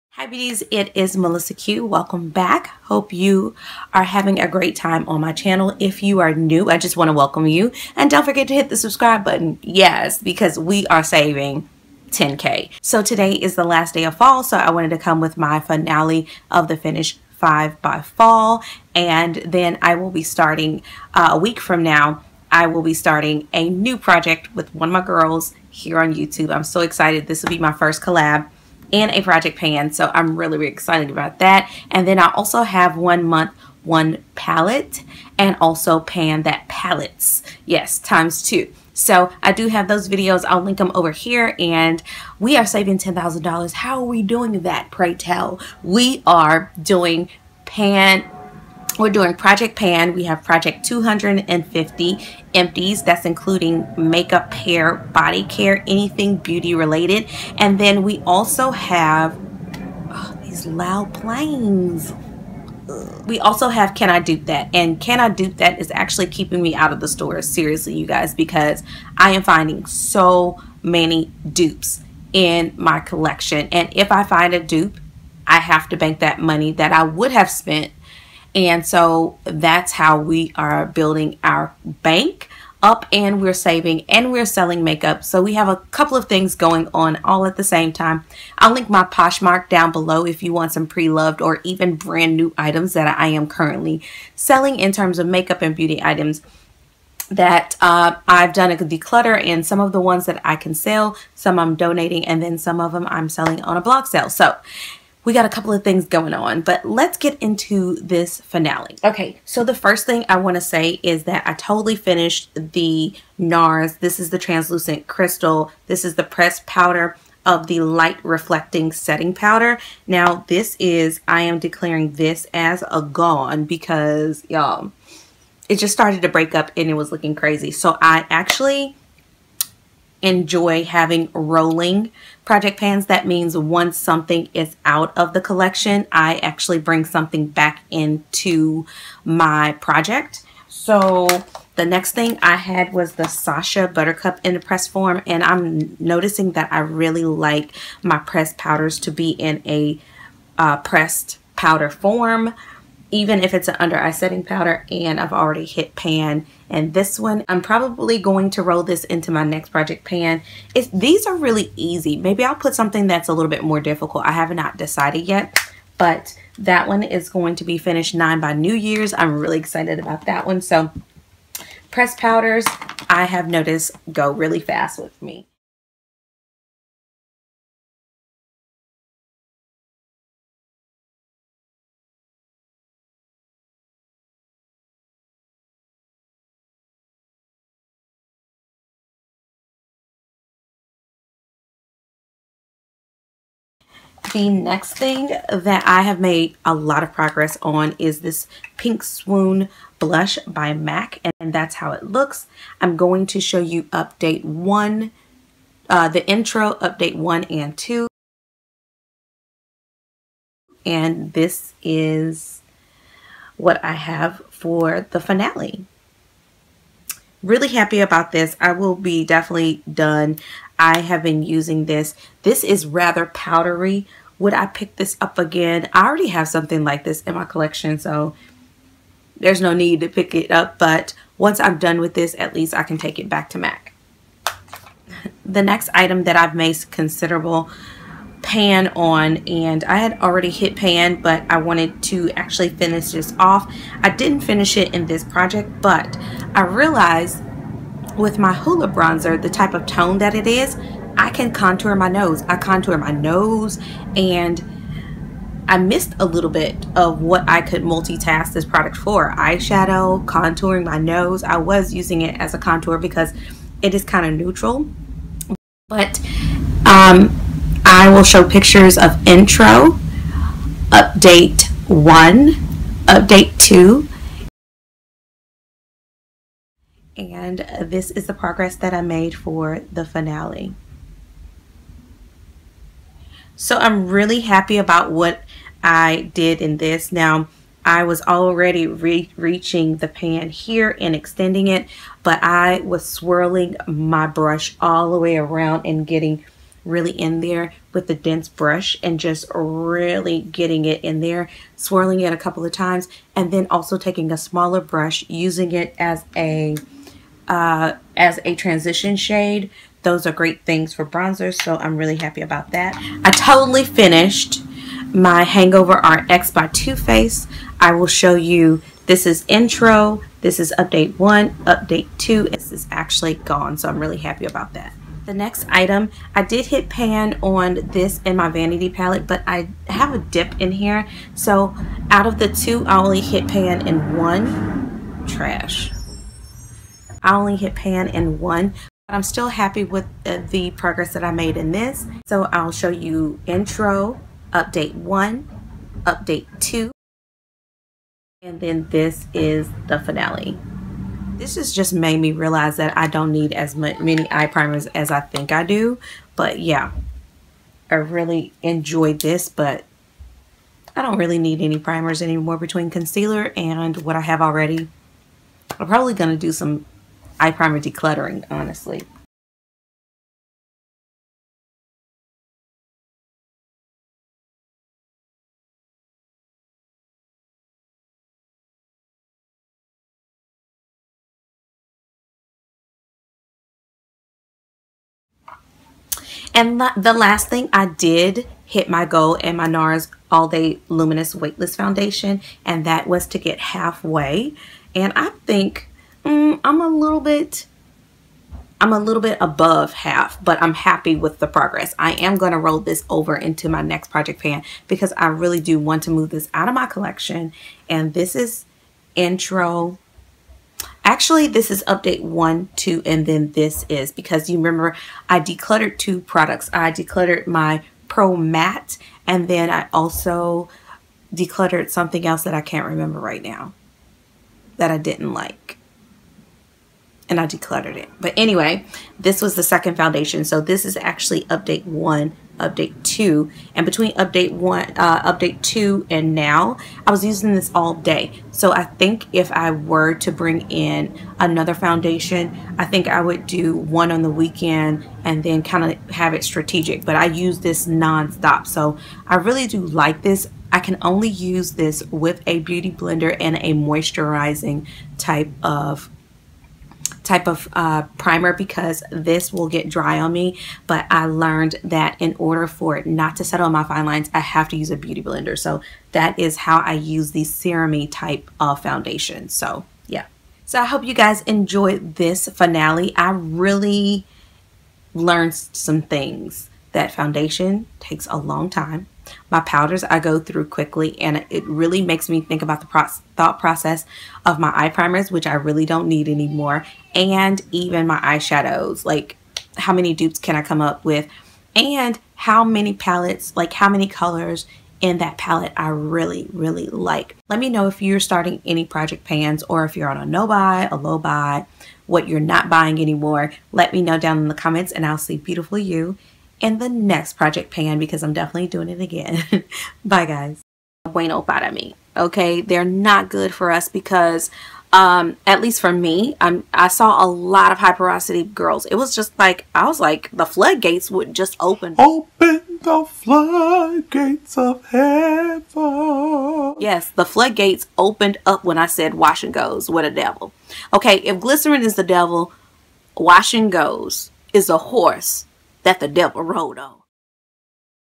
Finish 7 By Fall FINALE ~ Finish like a BOSS! | PROJECT PAN 2017 | MelissaQ
HEY Beauties and Curls! Today's video is the Project Pan FINALE to the Finish 7 By Fall challenge. Make sure to let me know the products you want to use up!

or ladyfykes2 on Poshmark

✔ PRODUCTS MENTIONED
Benefit Hoola Bronzer
NARS All Day Luminous Weightless Foundation
Too Faced Hangover RX
MAC Pinkswoon Blush
NARS Translucent Crystal Pressed Powder
Mica Beauty Eye Primer
Sacha Cosmetics Pressed Powder- Buttercup
MAC Fast Response Cream
L'Oréal Pro Matte Infallible Foundation

MelissaQ007

✔WRITE ME
Melissa Fykes